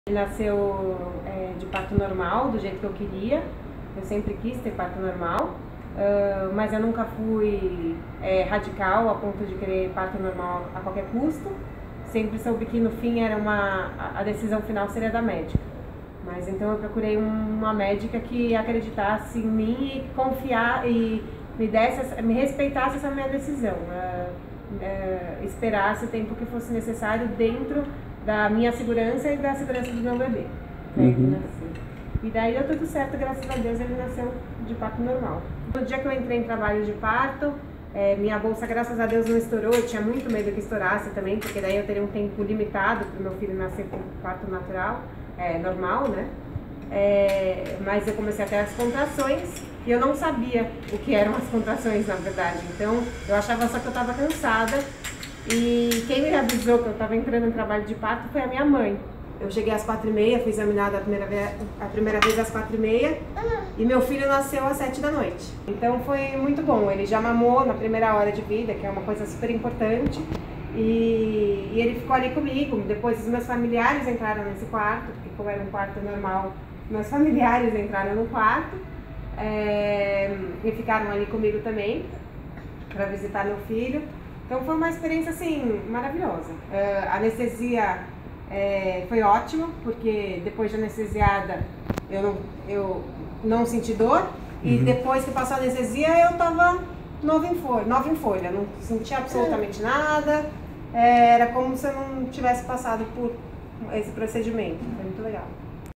Experiência do parto natural pode ser muito boa para mãe e filho
ESPECIAL WEB - 18.11.16: Adriana Foz, dentista, conta como foi sua experiência com o parto de seu primeiro filho. O Brasil é o campeão mundial de cesáreas no mundo. O Ministério da Saúde tem diversas campanhas para estimular nascimento natural, sem intervenções cirúrgicas que podem, por exemplo, aumentar a quantidade de partos prematuros.